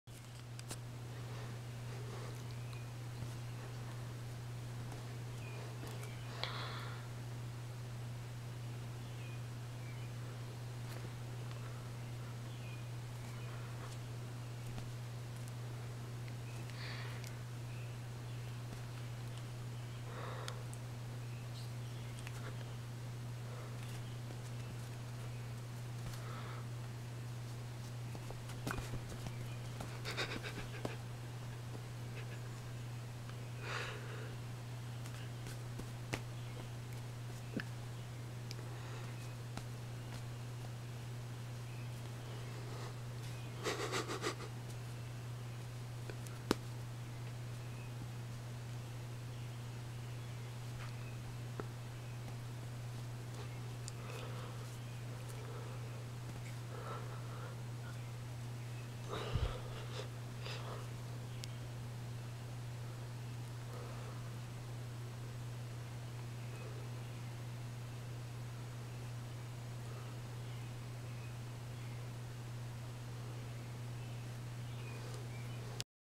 Playing with Zozo
Me playing with my cat Zozo. Now you know how I ended up with all these scratches on my hand/arms!